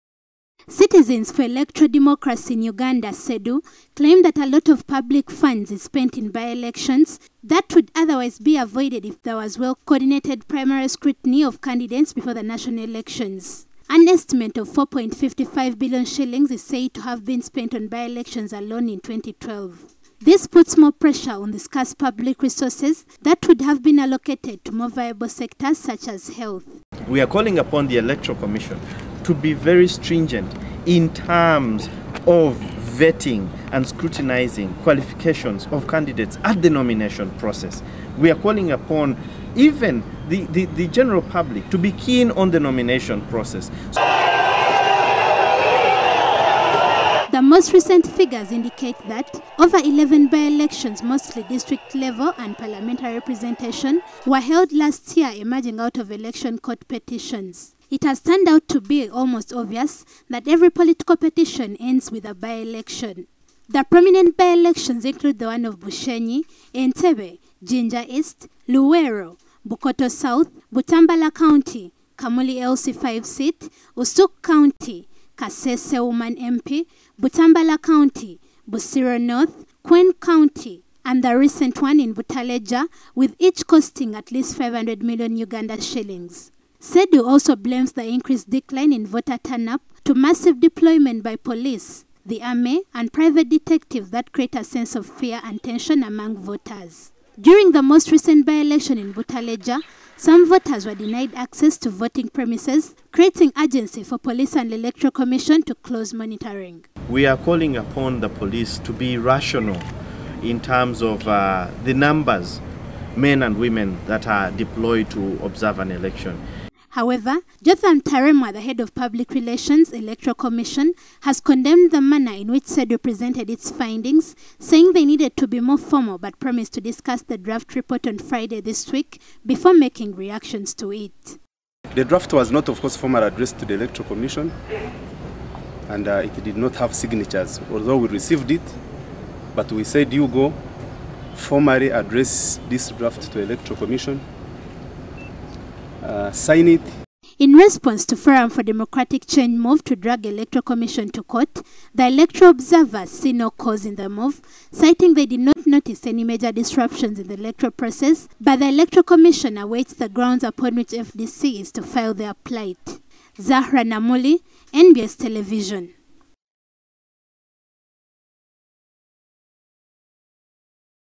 Election Observers Decry Multiple By Elections
Citizens for electoral democracy in Uganda (CEDU) a non governmental organization says the levels at which the courts of law award by-election ruling for different election petitions is worrying.